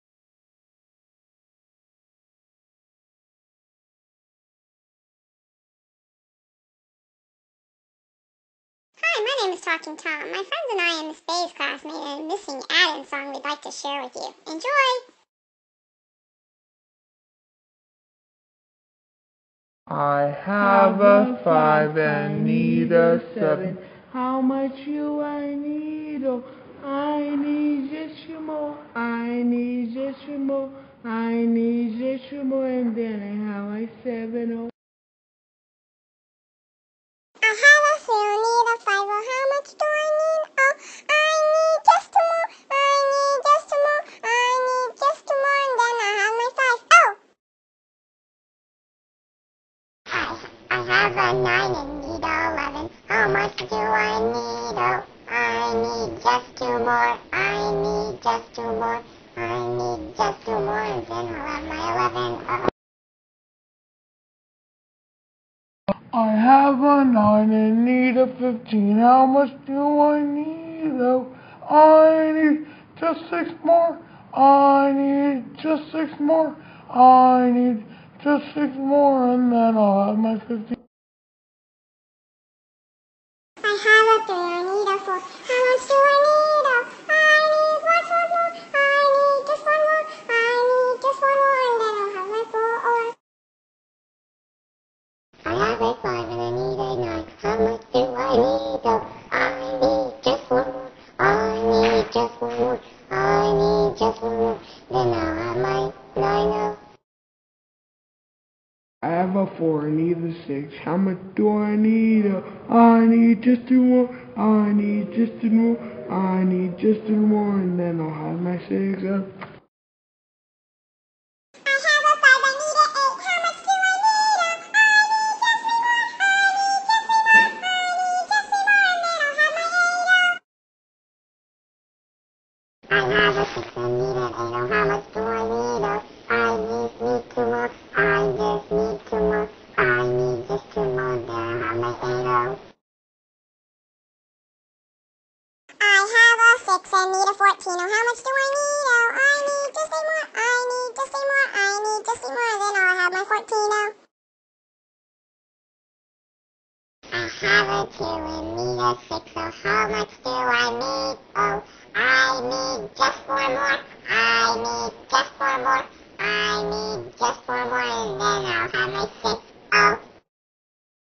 Bay's Missing Addend Movie for Computer.wmv
Ms. Bay's first grade class uses the iPads and the talking characters app to make a Missing Addend movie.